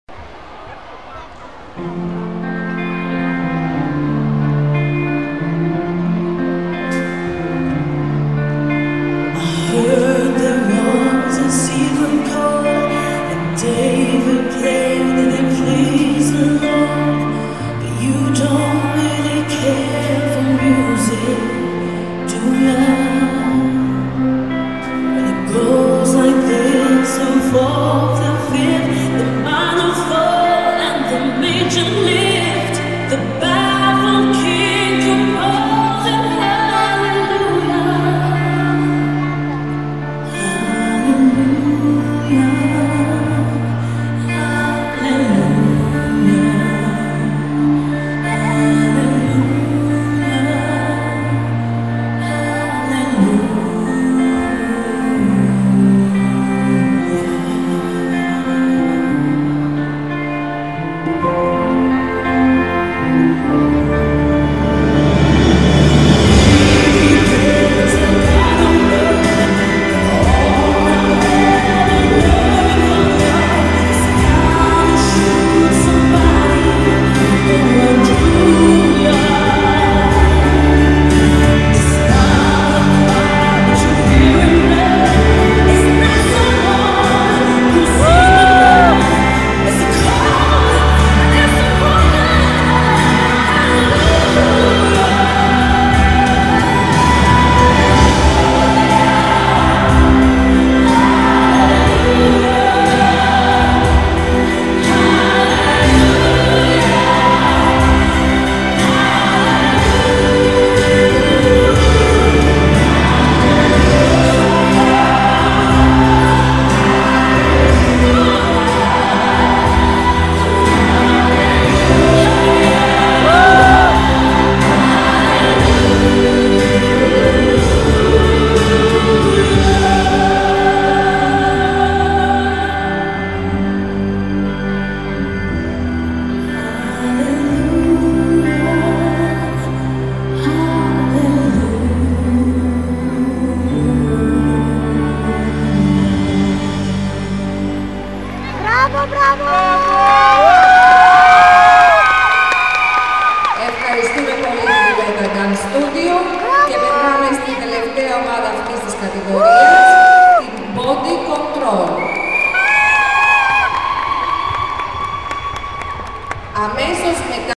Greek open 2013. Klasikos xoros sxoli PIROUETA
1 место в международных соревнованиях классических танцев. Школа ПИРУЭТА.
г. Салоники